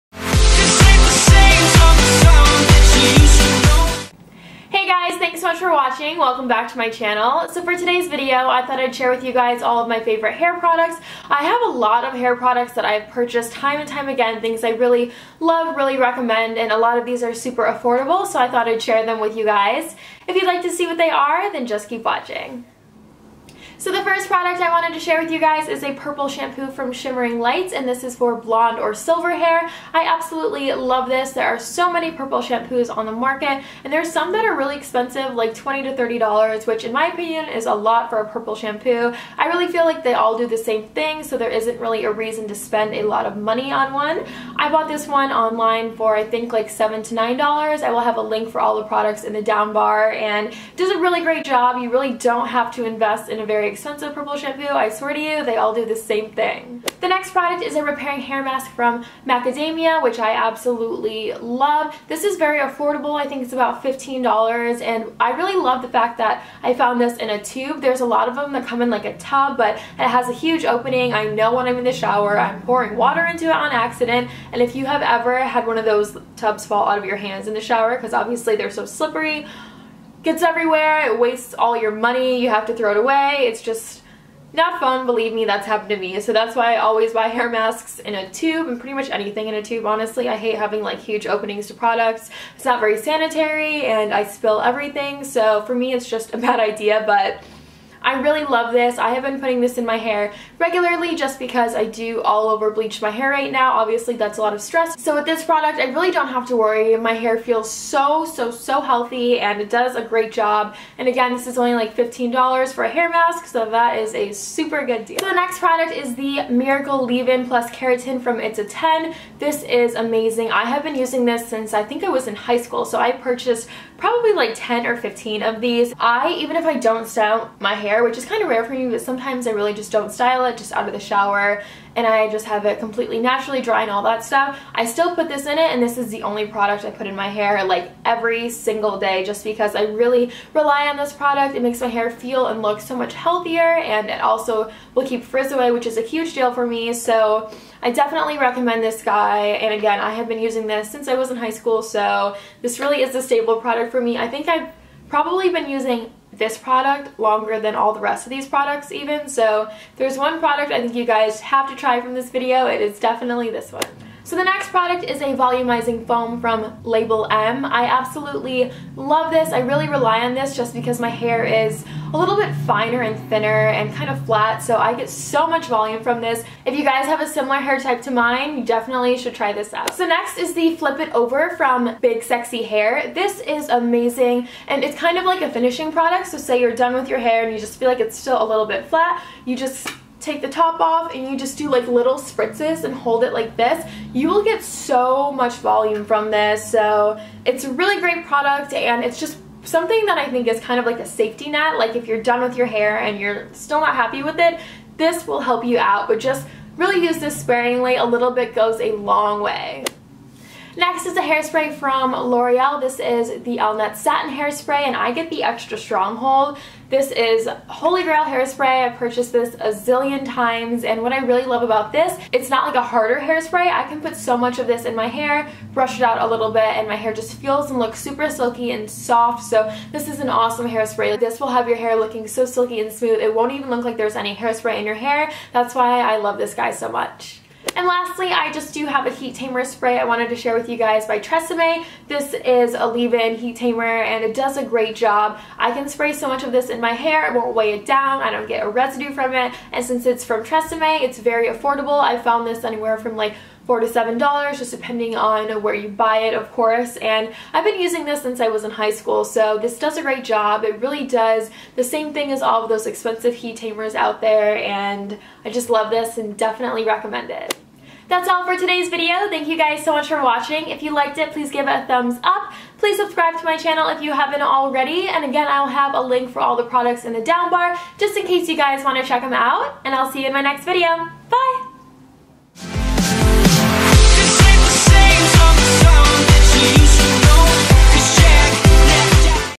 My Favorite Hair Products! | SimplyAlexandra321
Hi guys! For today's video I wanted to share with you all of my staple hair products! I highly recommend all of these, so if you want to try any of them out, I linked them below! XO, Alexandra